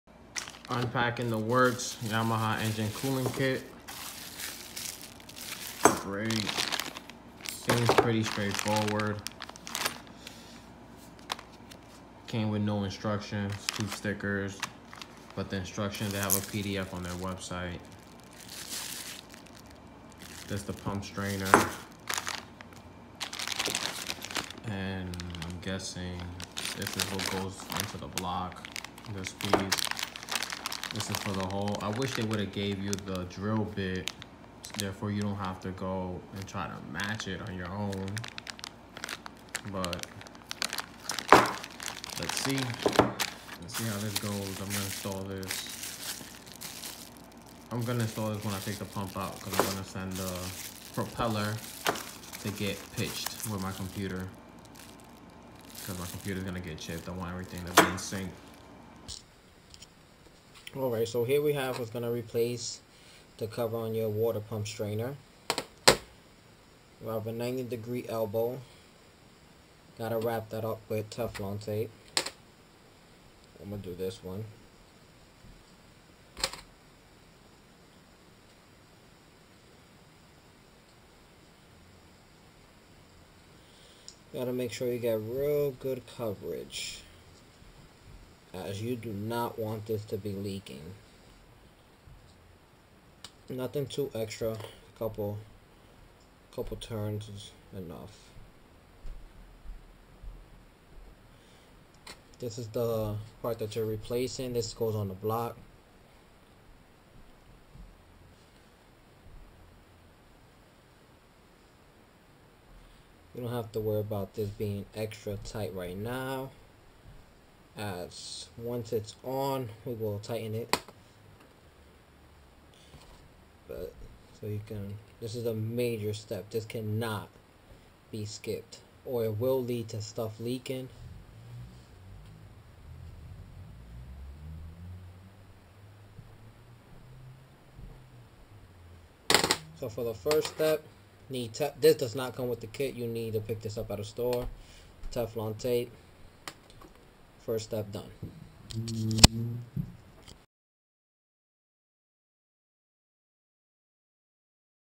Yamaha FX ho cruiser worx engine cooler upgrade installation part 1
Yamaha cooling kit upgrade for 1.8’high output engines with 155mm pump. This is an dyi and a must do if you are looking to chip your ecu and looking to rev the motor higher. The cooler you can keep the engine the better it is for the longevity of your machine.

I will be up loading a series of videos with the steps of how to complete this upgrade

This upgrade cost a total of 200 bucks but in my opinion well worth it